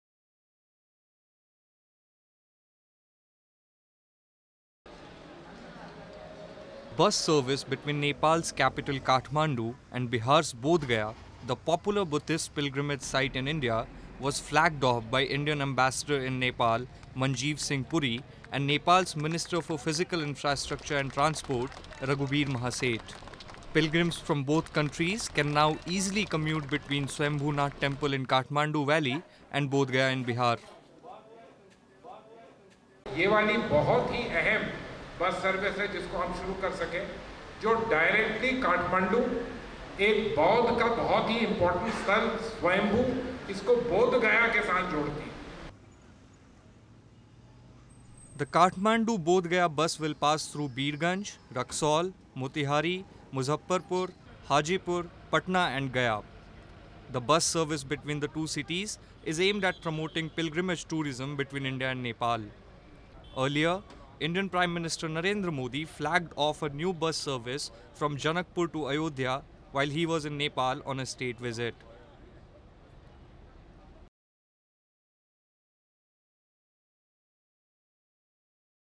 Bus service flagged off from Kathmandu to Bodh Gaya
Kathmandu (Nepal), Sep 14 (ANI): A bus service between Nepal’s capital Kathmandu and Bodh Gaya, the popular Buddhist pilgrimage site in India, was flagged off by Indian Ambassador in Nepal, Manjeev Singh Puri and Nepal’s Minister for Physical Infrastructure and Transport, Raghubir Mahaseth. Pilgrims from both countries can easily commute between Swayambhunath temple in Kathmandu valley and Bodh Gaya in Bihar. The Kathmandu - Bodh Gaya bus will pass through Birgunj, Raxaul, Motihari, Muzaffarpur, Hajipur, Patna and Gaya. The bus service between the two cities is aimed at promoting pilgrimage tourism between India and Nepal. Earlier, Indian Prime Minister Narendra Modi flagged off a new bus service from Janakpur to Ayodhya, while he was in Nepal on a state visit.